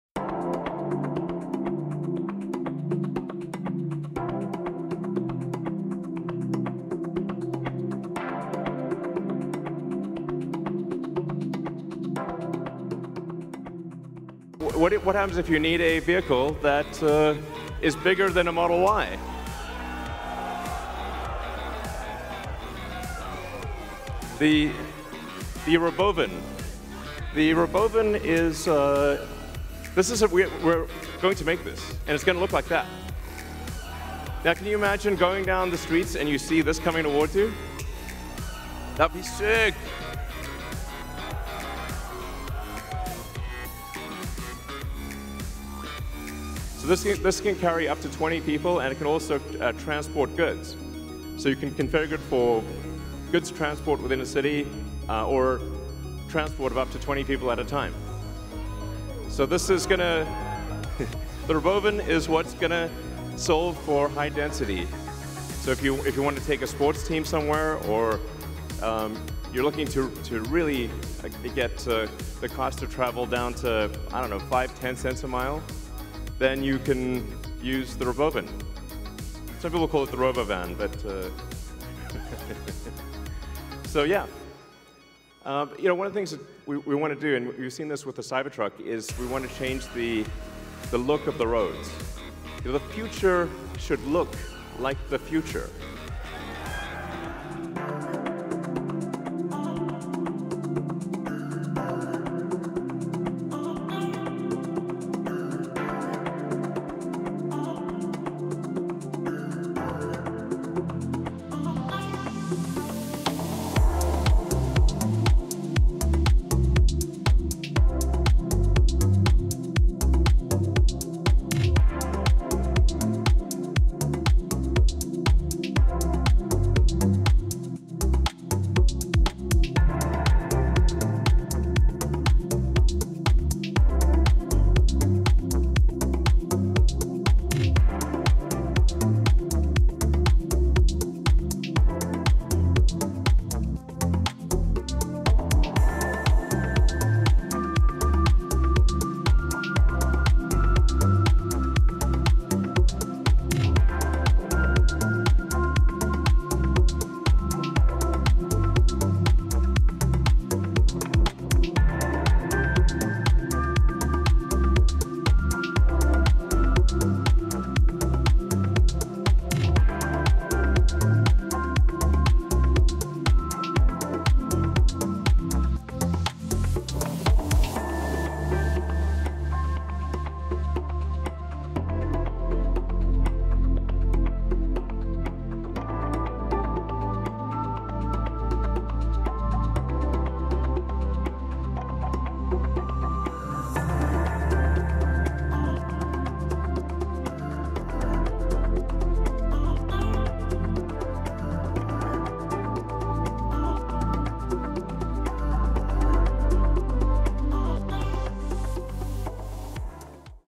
Tesla ROBOVAN Reveal | Iron Man-like Design !
Elon Musk revealed the Tesla ROBOVAN during the We, Robot event !

Autonomous transport for groups or goods, from mass transit to construction to food service.

Source : Tesla